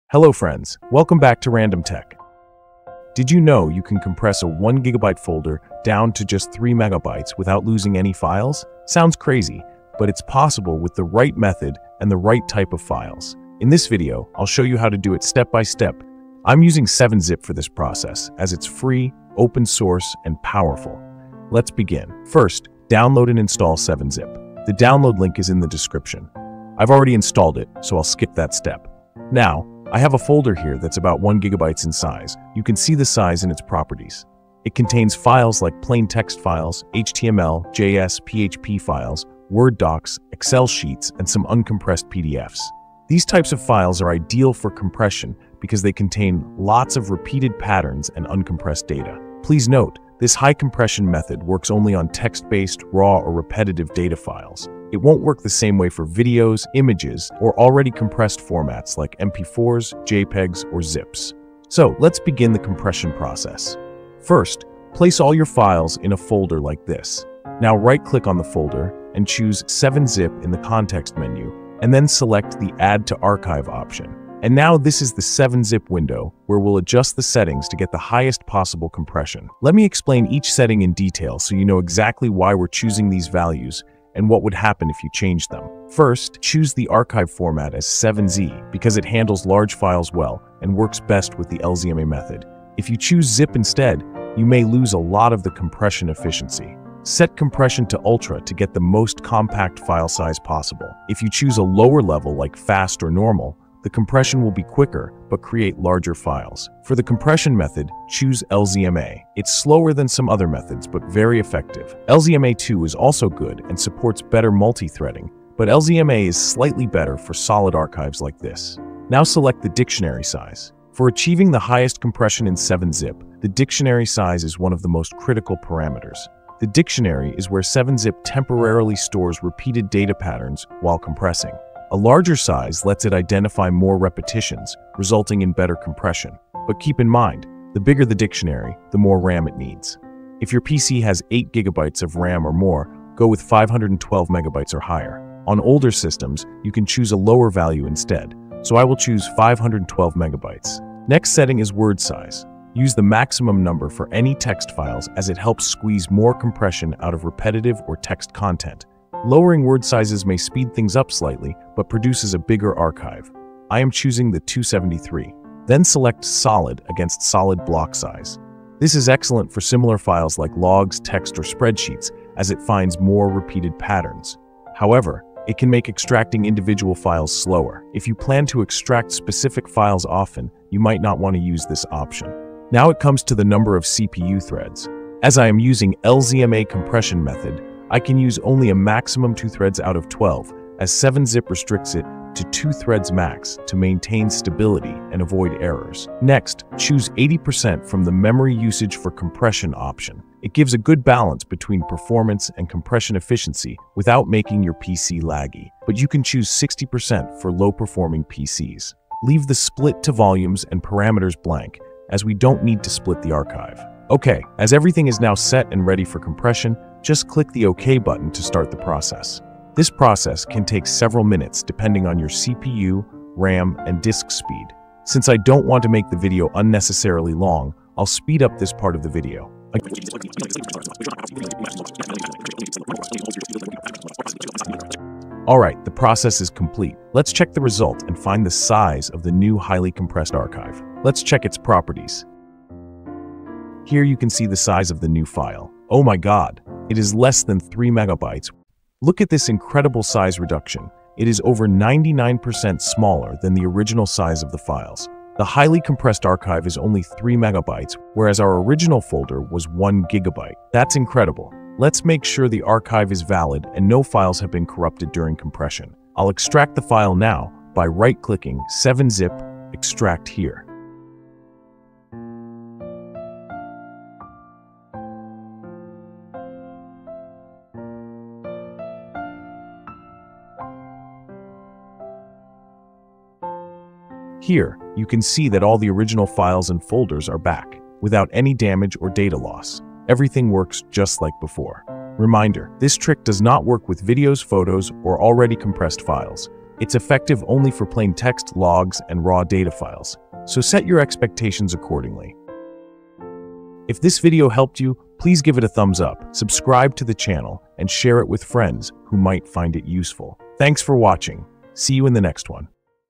How To Compress 1GB File to 3MB With 7-Zip | With Proof | Latest 2025
Here is the updated video on how to compress a 1 GB file to just 3 MB or less using the 7-Zip file archiver. In this video, we show a step-by-step guide for 7-Zip's highest compression settings to achieve absolute file compression without losing any data.

7-Zip is one of the best and powerful file archives that allows you to compress files to an extreme level. In some cases, you can compress a 1GB file or folder to just 5 MB, or even less than 3 MB. But to achieve this level of highest file compression, you have to apply proper 7 Zip high compression settings. Watch this step-by-step guide completely to learn the 7-Zip best settings and compress a 1 GB file to just 3 MB or less.
Please note that this high-compression method works on some particular file like text-based files, uncompressed PDFs, raw, or repetitive data files, etc. It won’t work the same way for videos, images, or already compressed formats such as MP4s, JPEGs, or ZIPs.